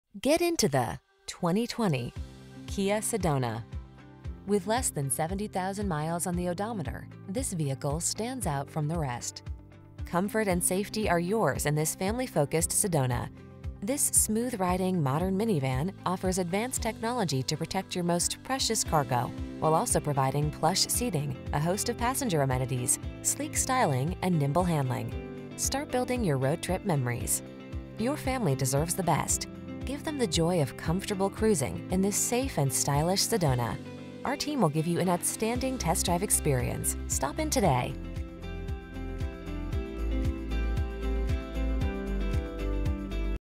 2020 Kia Sedona Lewisville, Dallas, Carrollton, Richardson, TX NX017656A
Snow White Pearl Used 2020 Kia Sedona available in 1547 S. Stemmons Freeway, Lewisville, Texas at Lone Star Toyota. Servicing the Lewisville, Dallas, Carrolton, Richardson, TX. area.

1547 S. Stemmons Fwy